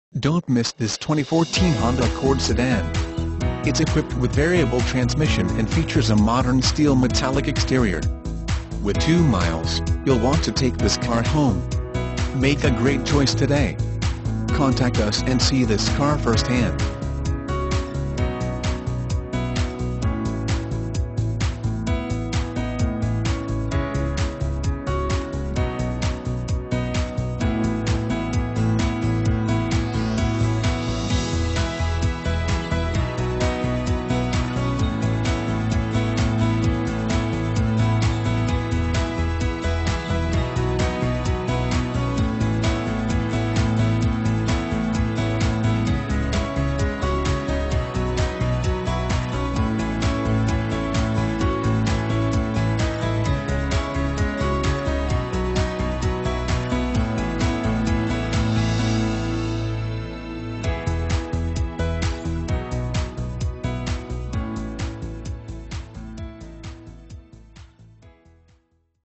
2014 HONDA ACCORD SEDAN Huntington, WV H14359
2014 HONDA ACCORD SEDAN Huntington, WV
Stock# H14359

5210 US Rt. 60 E.

Don't miss this 2014 Honda Accord Sedan. It's equipped with Variable transmission and features a Modern Steel Metallic exterior. With 14 miles, you'll want to take this car home. Make a great choice today.